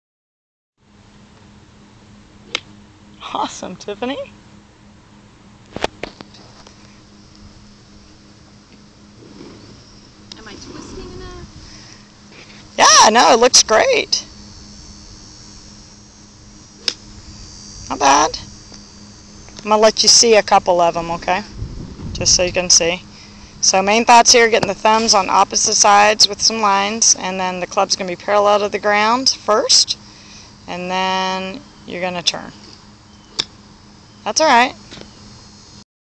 Tiffany P/full swing (9/12/11)
set up: grip-natural arm hand w/thumbs on opposite of grip, push grip down so handle under forearms
swing: club parallel to ground and toes,then turn back to target (watch that arms want to pull back too far)